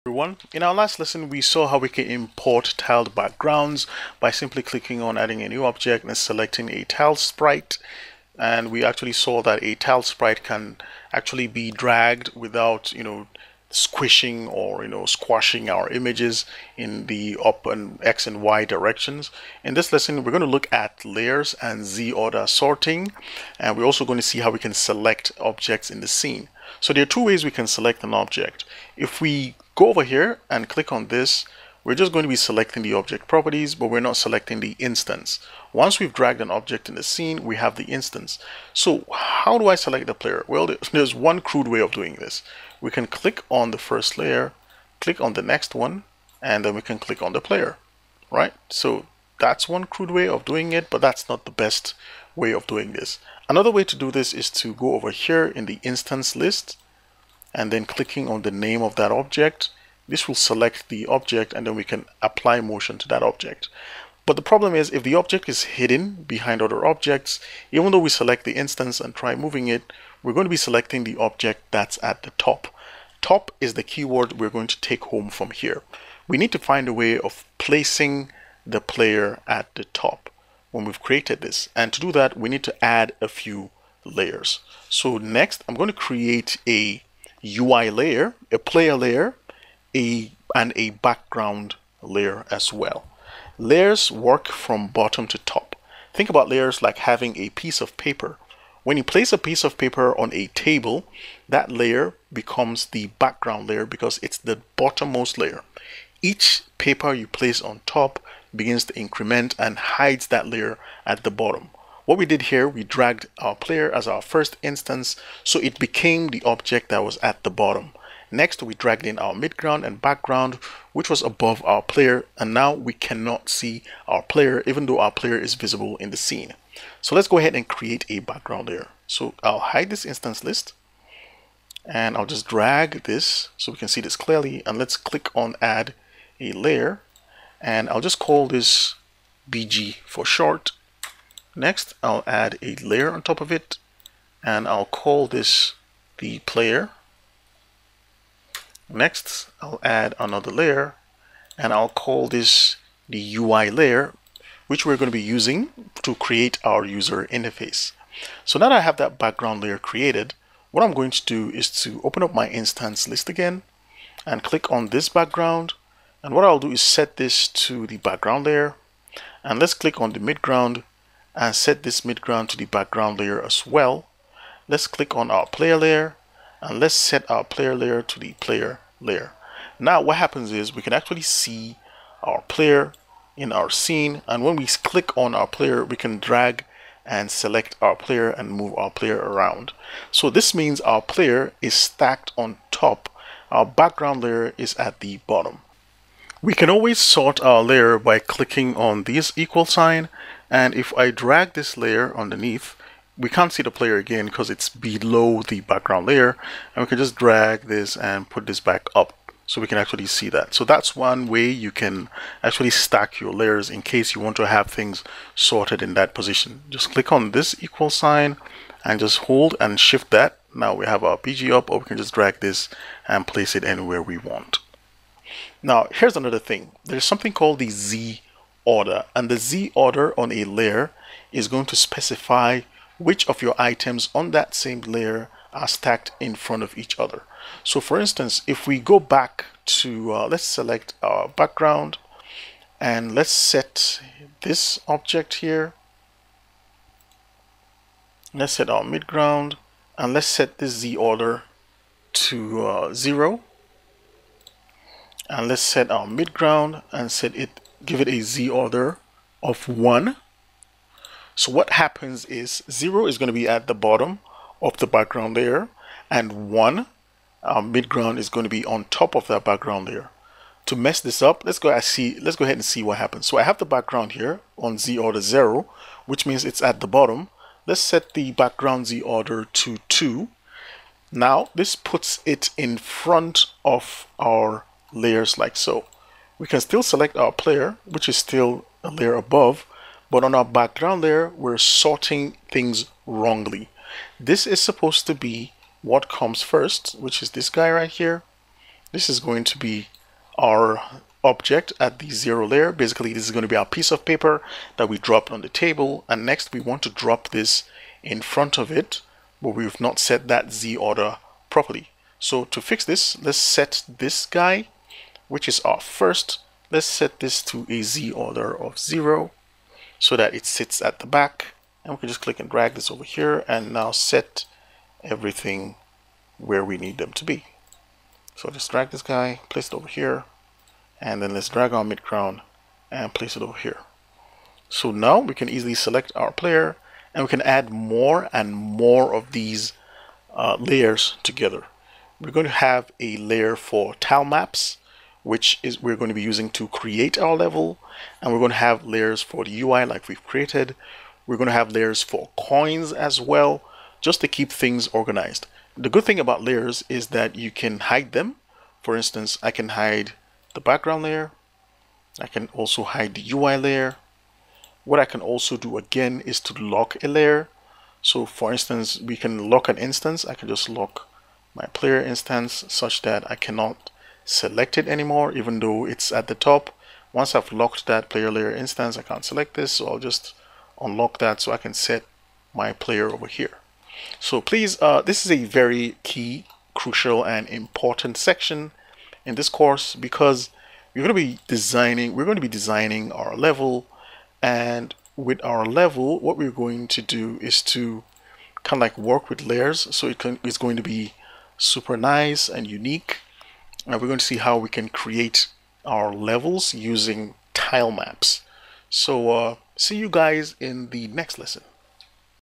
GDevelop Tutorials 09 Create a 2D Game - 09 Working with Layers
Layers allow game developers to properly map their players, sort them between game objects, add effects, and more. Let's learn some common layer techniques to make it easier when planning your game.